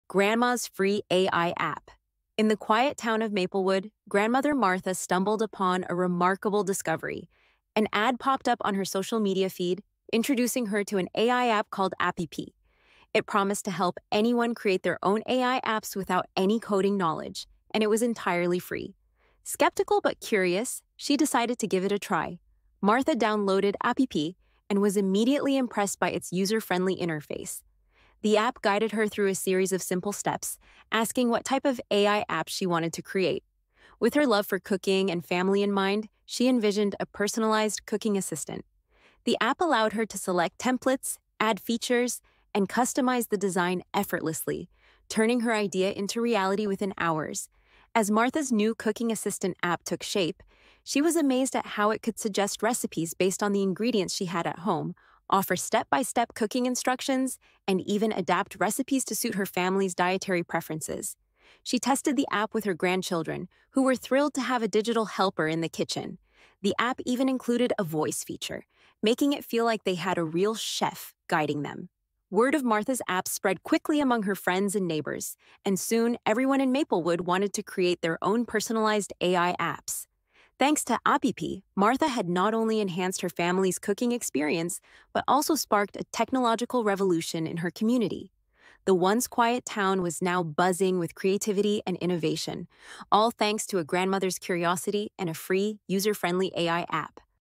Grandma created a Cooking Assistant App - using Appy Pie
In the quiet town of Maplewood, grandmother Martha discovered an incredible AI app called AppiPie, which allows anyone to create their own AI apps without any coding knowledge, and it’s entirely free. Curious but skeptical, Martha downloaded Appy Pie and was amazed by its user-friendly interface and simple steps. She created a personalized cooking assistant app that suggests recipes based on ingredients, offers step-by-step instructions, and adapts to dietary preferences. Her grandchildren loved the digital helper, and soon the whole town of Maplewood was creating personalized AI apps. Thanks to Appy Pie, Martha sparked a technological revolution in her community, enhancing cooking experiences and spreading innovation.